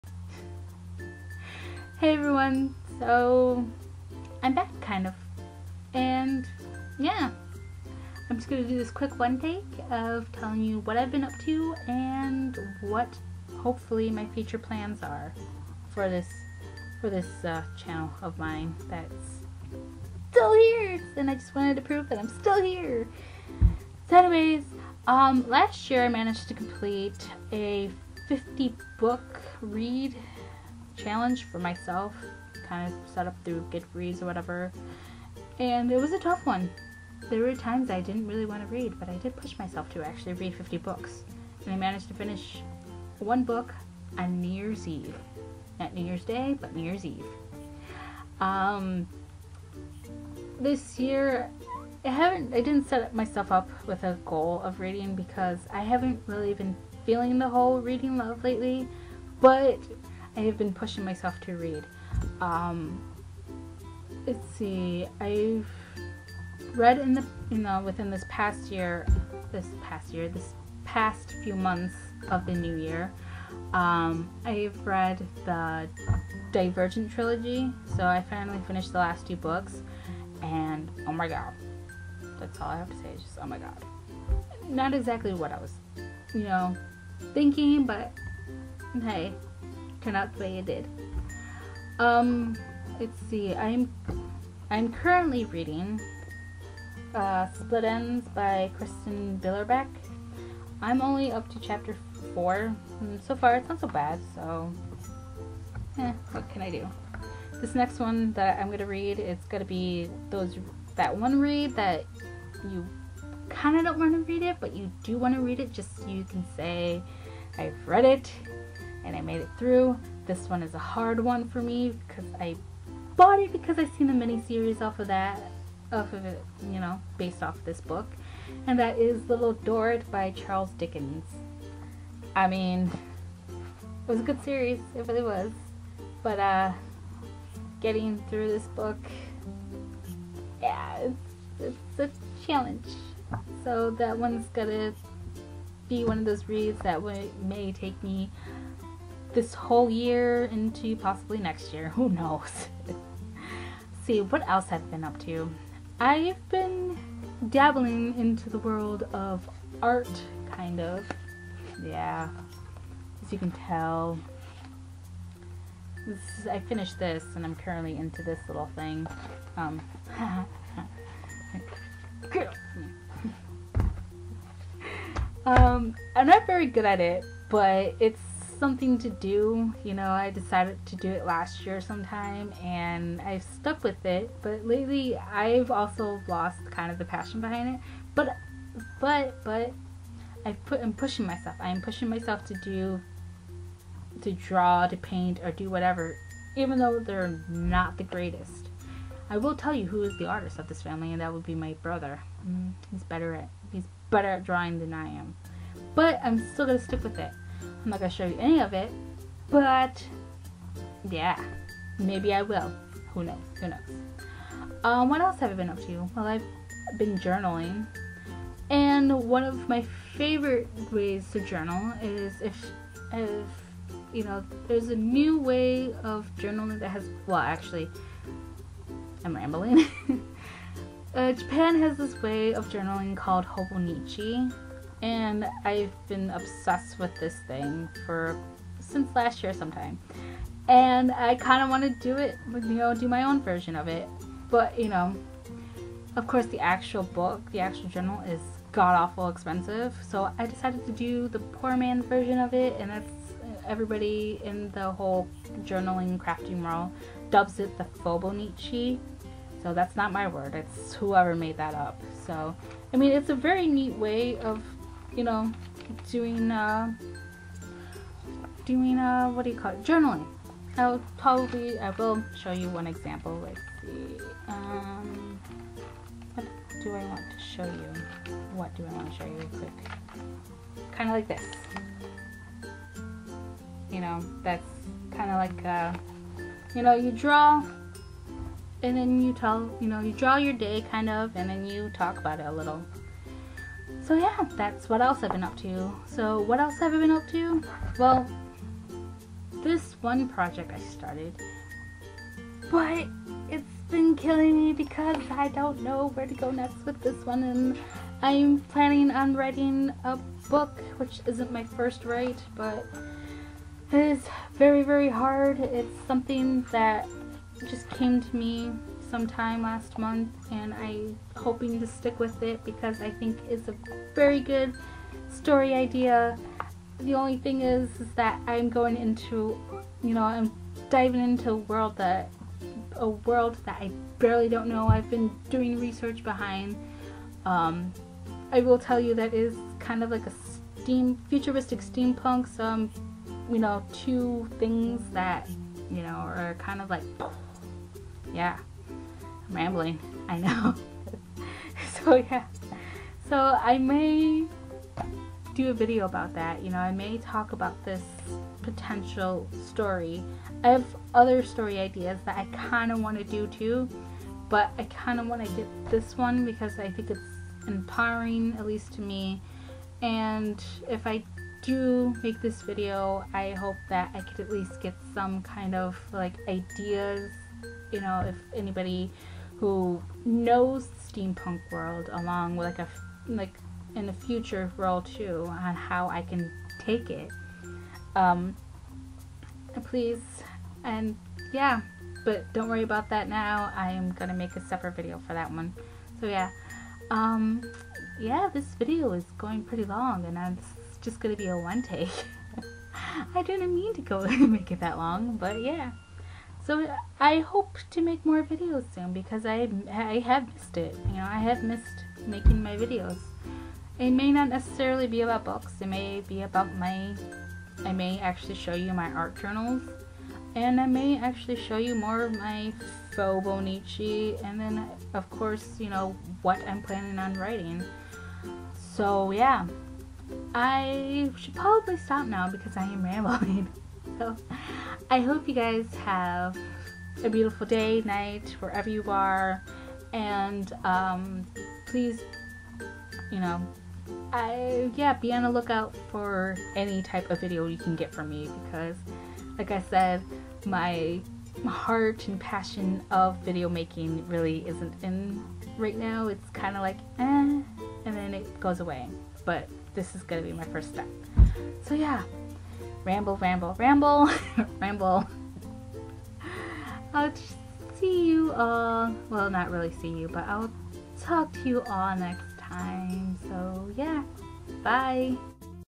I'm Back Again.......Hopefully!!!
So basically I'm back maybe...hopefully. I explain what I have been up too and what I hope to do.